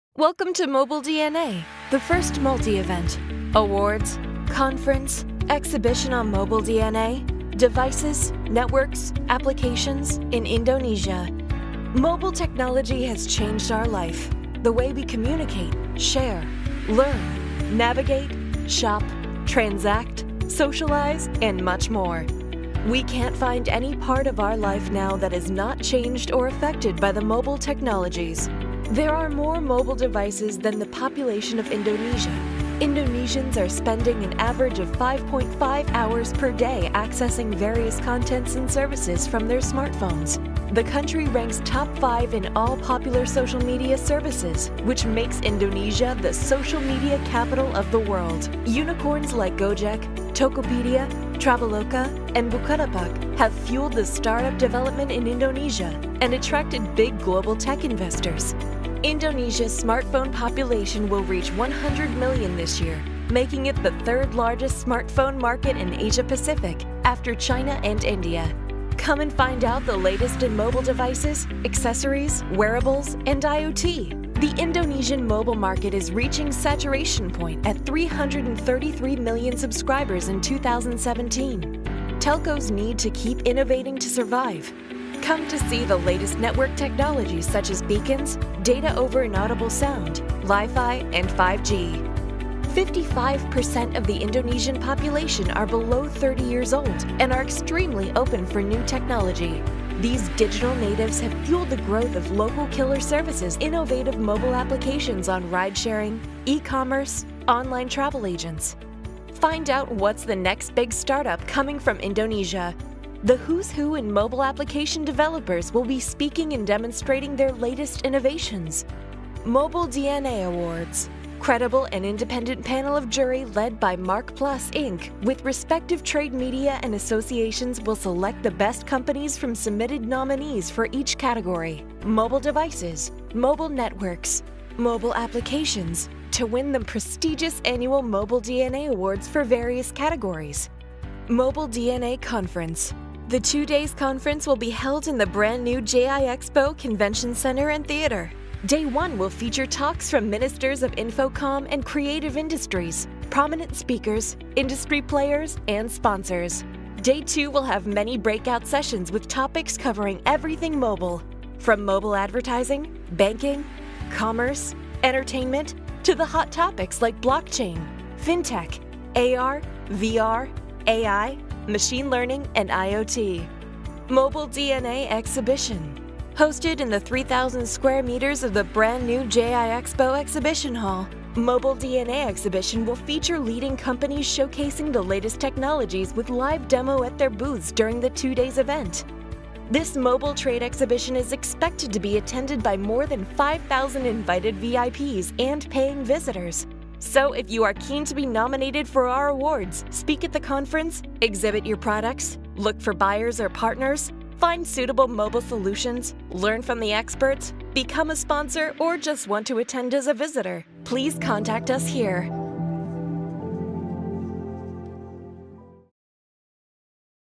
Mobile DNA 2018
COME AND JOIN

MOBILE DNA EXPO 2018
The 1st Multi event - Awards, Conference, Exhibition on Mobile DNA (Devices, Network, and Application) in Indonesia

Location: Jakarta International Expo (JIExpo), Kemayoran, Jakarta, Indonesia
Date: 14-15 November 2018
Time: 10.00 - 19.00 WIB (Western Indonesia Time)

Supported by:
- inTouch
- Kompas
- MarkPlus, Inc
- More
- Kejora
- Metro TV

Mobile Technology has changed our life.
The way we communicate, share, learn, navigate, shop, transact, socialize and much more.
We can't find any part of our life now that is not changed or affected by the mobile technologies.

Video uploaded by:
Kendro Hendra from inTouch